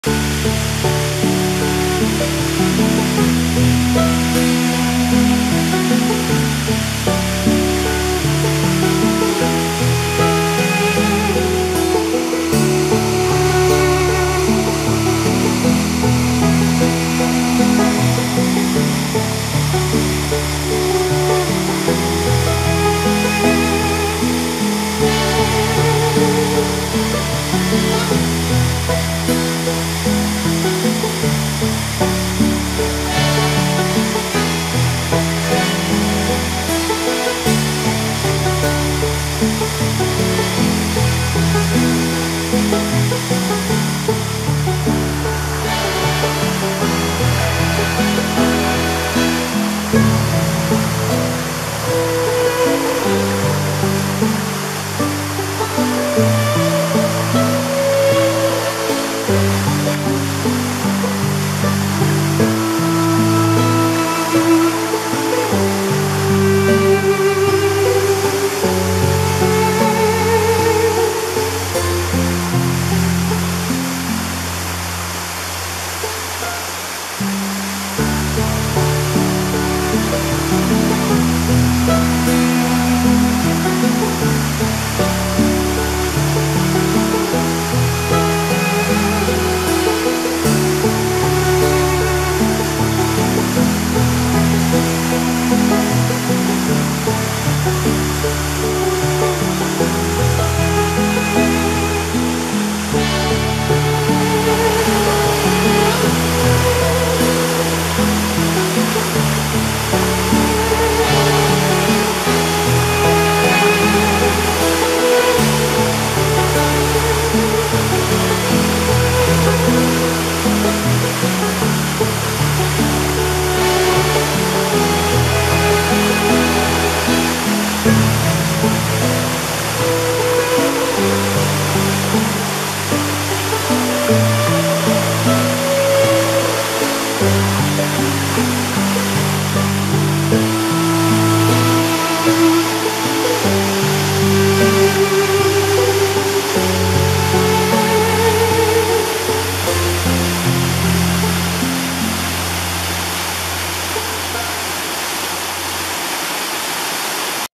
Soco Falls -Hwy 19- Qualla Indian Reservation near Maggie Valley, NC
Double falls from two separate tributaries. Easy walk from gravel parking area on Hwy 19 near Maggie Valley, NC
Probably no more than 300 to 500 Ft to viewing platform but to view both falls you need to hike to the bottom which is narrow and steep with ropes along the way to help. Really not that bad, well worth it.